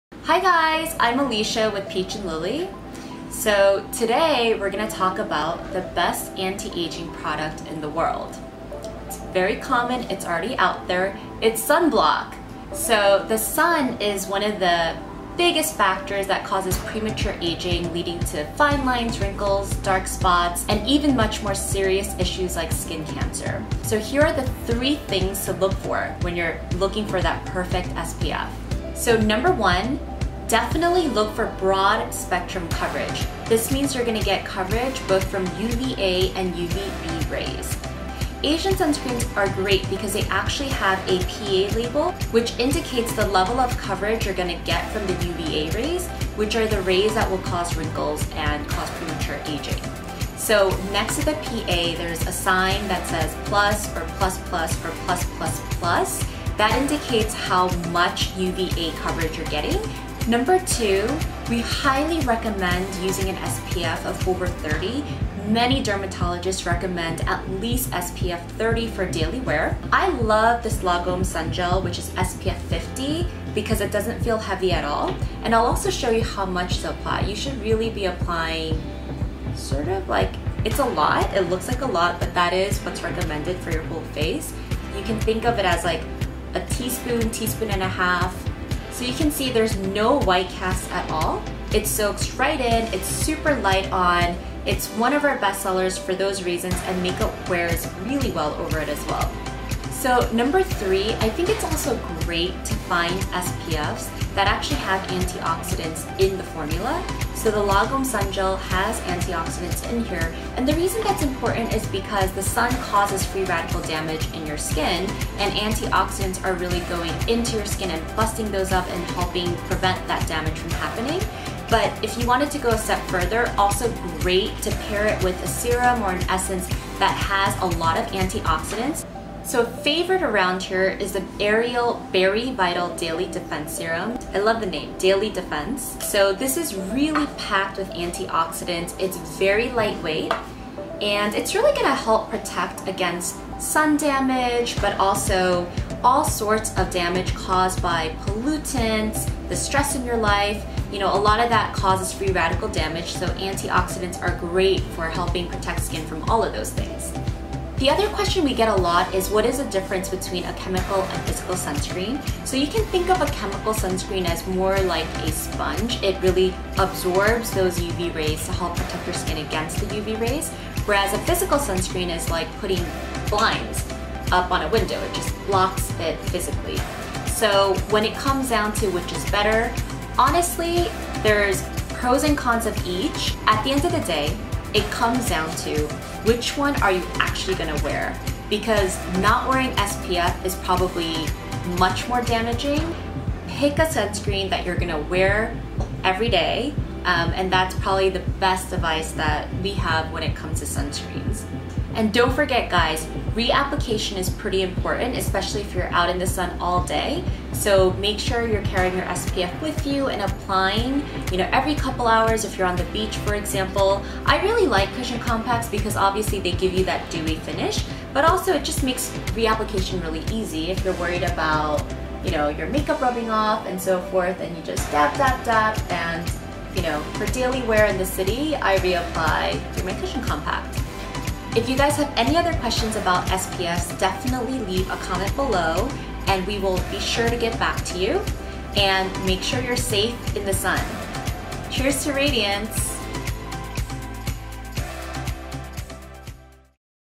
3 Things You Need To Know Before Buying Sunscreen | Peach & Lily
Peach & Lily founder & licensed esthetician, Alicia Yoon, breaks down the 3 things you need to look for in the best anti-aging product in the world — sunscreen!

The content seen in this video is provided only for education purposes and is not intended to be a substitute for professional medical advice, diagnosis, or treatment.